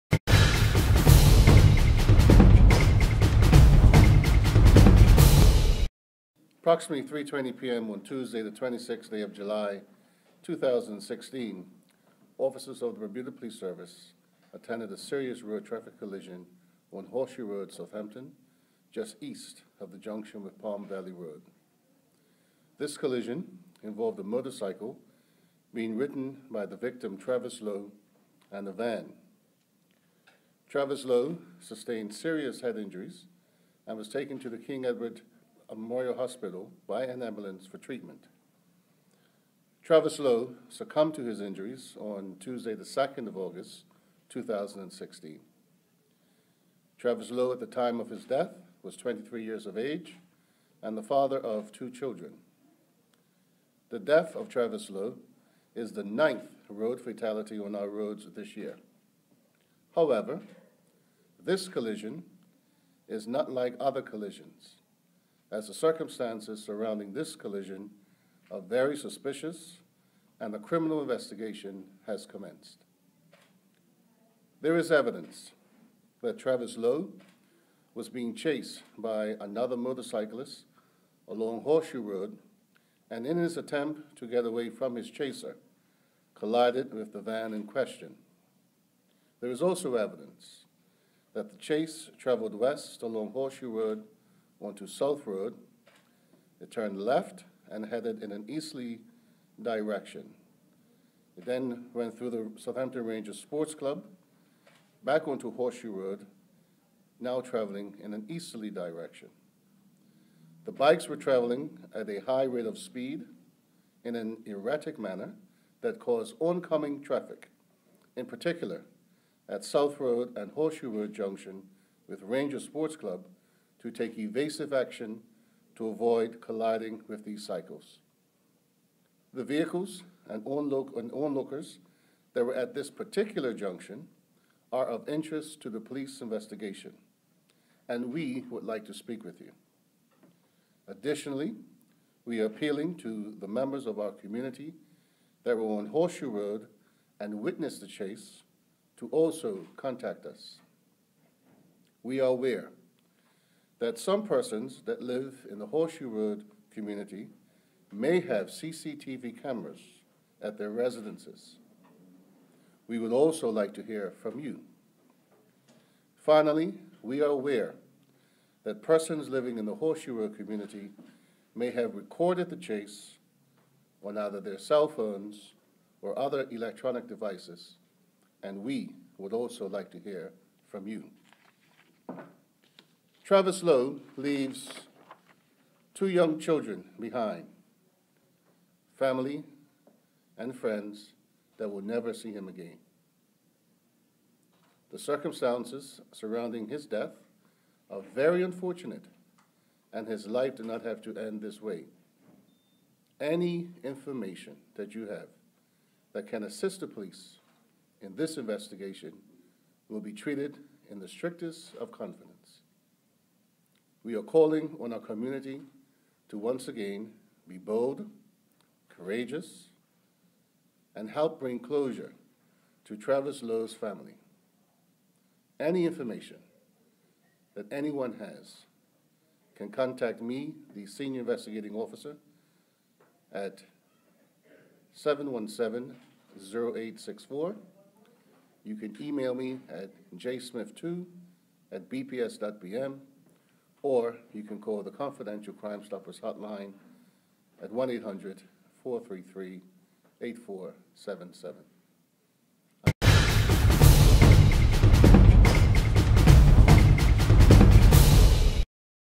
Police On Investigation Into Death of Travis Lowe, August 9 2016
| Bermuda | The circumstances surrounding the collision that resulted in the death of Travis Lowe “are very suspicious and a criminal investigation has commenced,” the police said, adding that “there is evidence that Travis Lowe was being chased by another motorcyclist along Horseshoe Road and in his attempt to get away from his chaser collided with the van.”